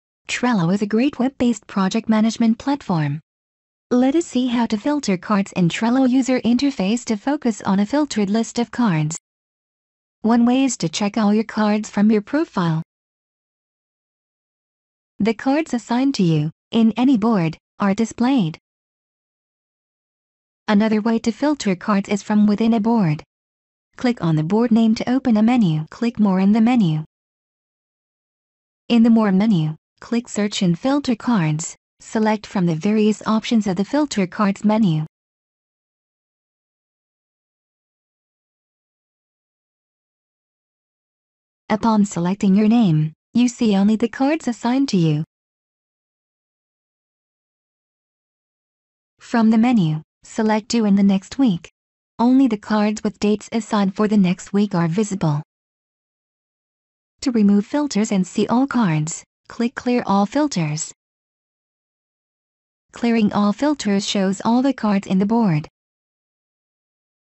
Filtering cards in Trello for more focus on specific tasks
To work on specific tasks, you may want to focus on those and remove the clutter from your Trello board. Here's how to do it.